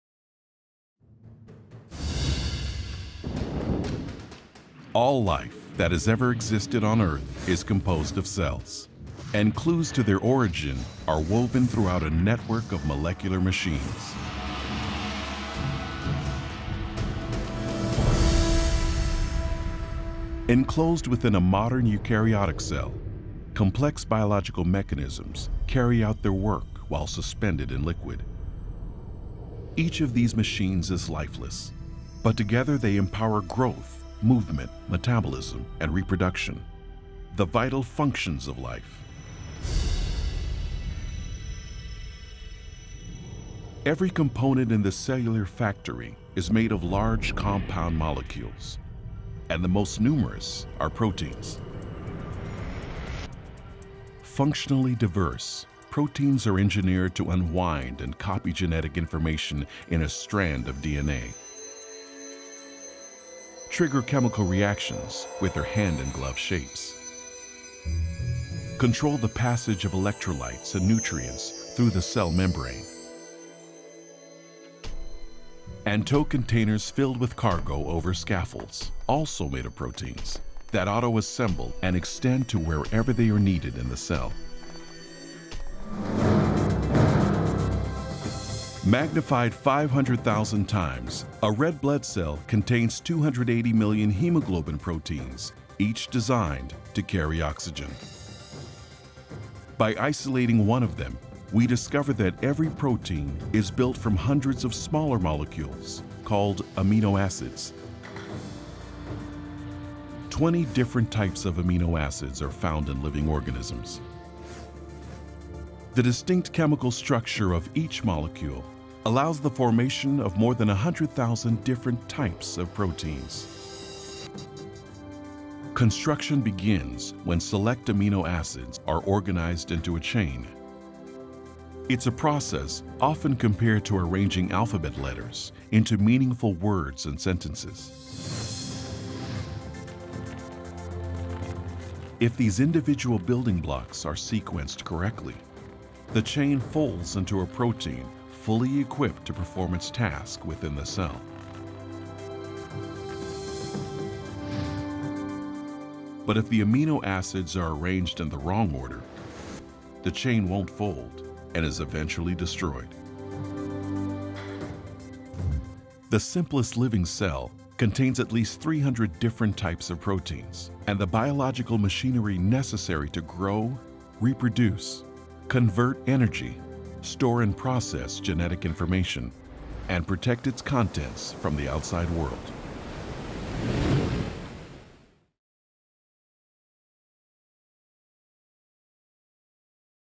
ORIGIN - How to Build a Cell
Take a dynamic trip into a living cell to discover how the biological machinery required for life is engineered and constructed.